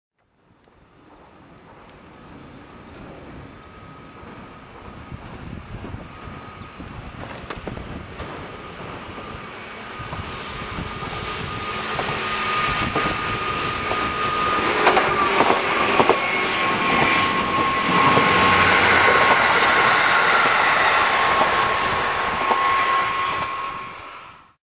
JR東海 キハ75回送 東海道本線西小坂井～豊橋
2006年7月に豊橋まで走ったキハ75です。豊橋到着後すぐに折り返してきたのですが、一体何の列車だったのかあいにく記憶にありません...。その後、日車入出場や飯田線の臨時などの関係で何度もこの区間を走行していますので、今では珍しくはなくなりましたね。飯田線の下地駅からの撮影です。